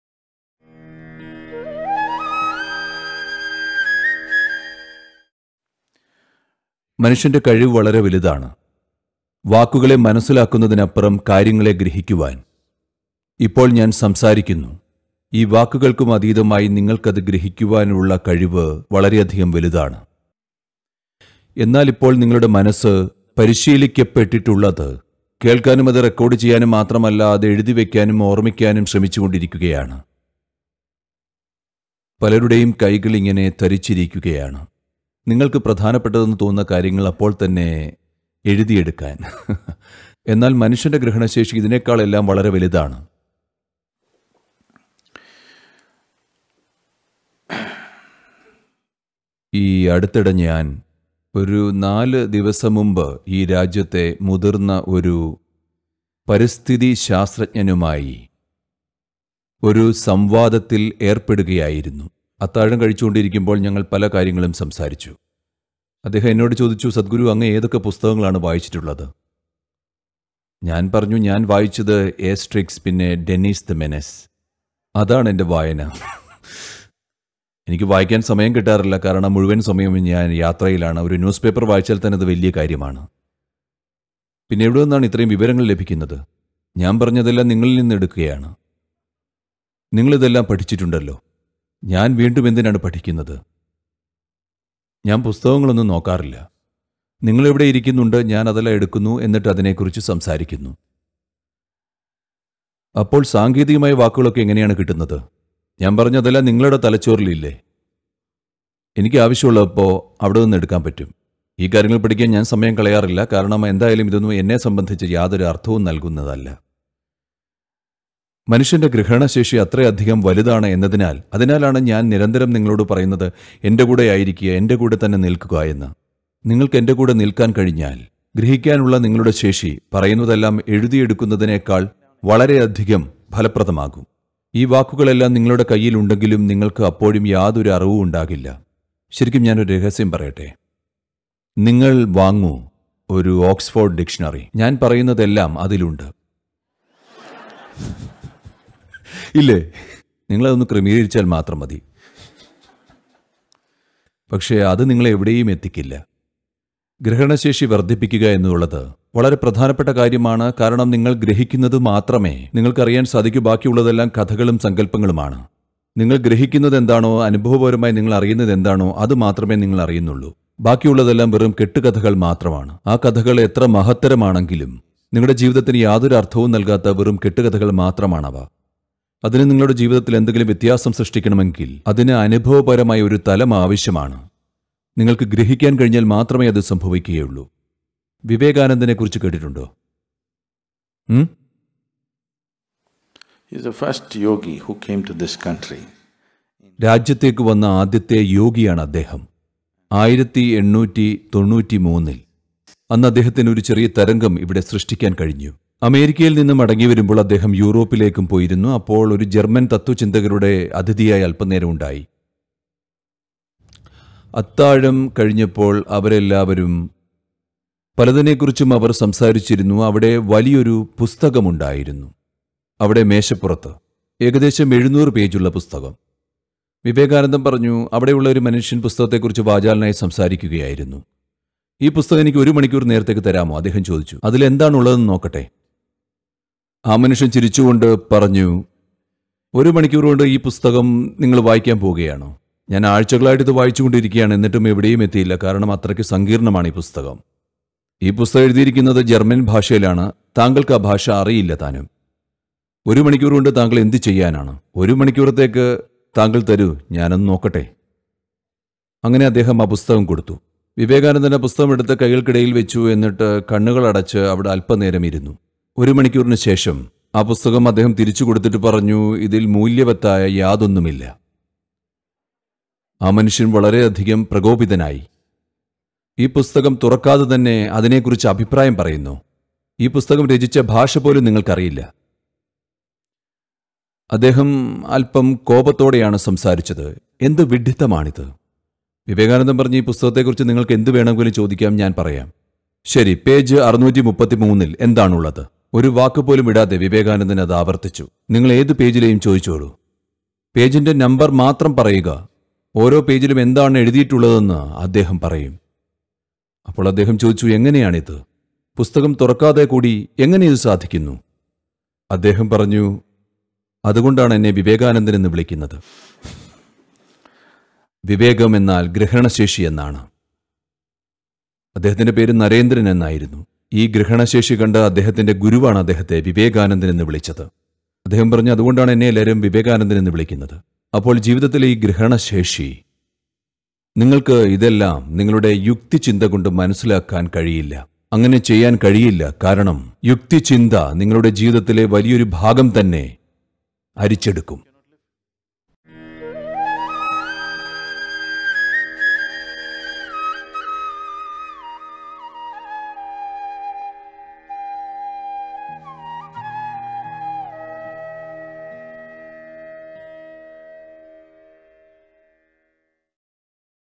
സ്വാമി വിവേകാനന്ദൻ്റെ യുക്തിക്കുമതീതമായ ഗ്രഹണ ശേഷി | Swami Vivekananda mystical power
"യുക്തിചിന്ത... നിങ്ങളുടെ ജീവിതത്തിലെ ഒരു വലിയ ഭാഗം തന്നെ അരിച്ചെടുക്കും".. സദ്ഗുരു പറയുന്നു, മനുഷ്യന്റെ ഗ്രഹണ ശേഷിക്ക് യുക്തിചിന്തയ്ക്കുമതീതമായി സഞ്ചരിക്കാൻ കഴിയും